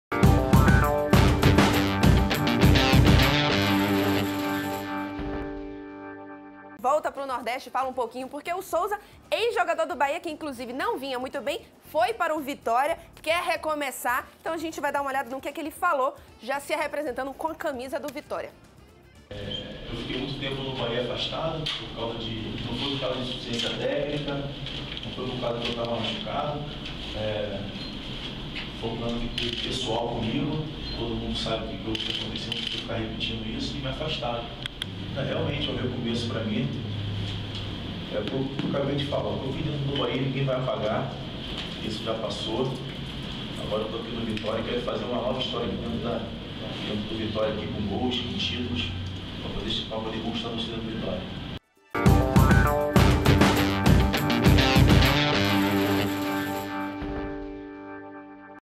Vitória apresenta o atacante Souza
O ex-Bahia comenta sobre a mudança de time. O rubro-negro aproveitou para apresentar outro reforço, o meia Hugo.